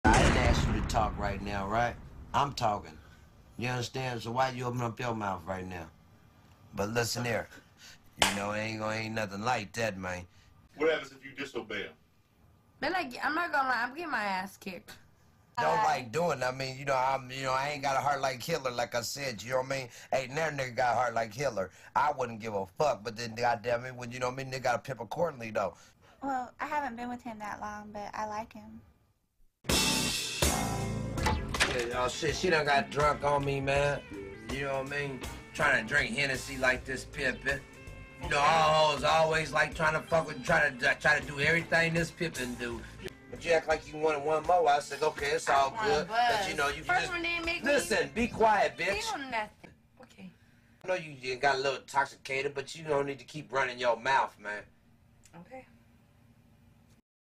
Becky Lynch Pimping 101 Skit
RAW has booked Seth Rollins to look like a bitch. This is a skit reflecting the Becky Lynch / Seth Rollins / Maria Kannellis storyline of July 2019.